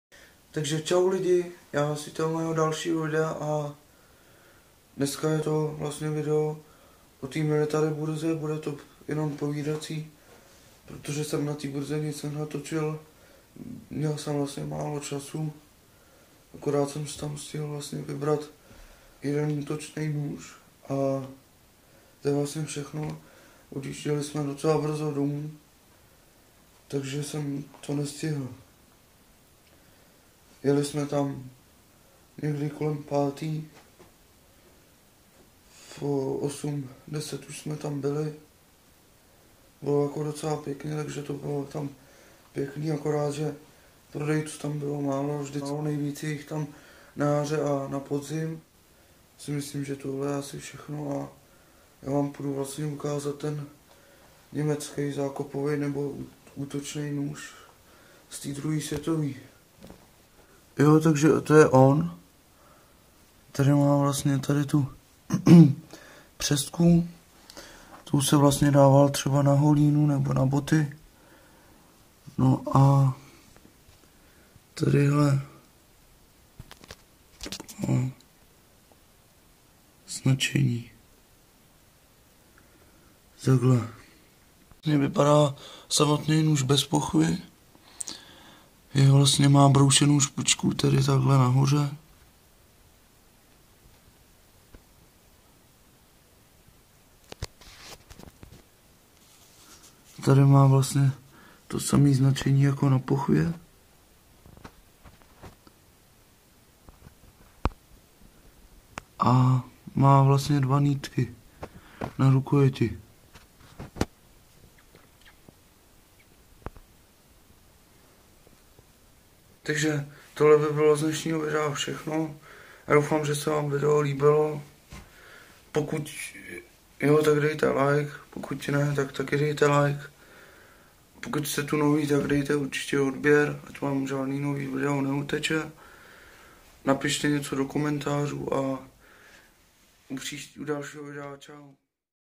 MILITARY BURZA PRAHA #2 | URBY
Čau lidi, vítám vás u mého dalšího videa a dneska jsem natočil o military burze v Praze. Bohužel jsem na ni nic nenatočil protože bylo málo času, byly jsem tam jen chvíli. Koupil jsem si německý útočný nůž z 2.sv. války. Bude video o mé nové vitríně, těšte se!!! U dalšího videa čau!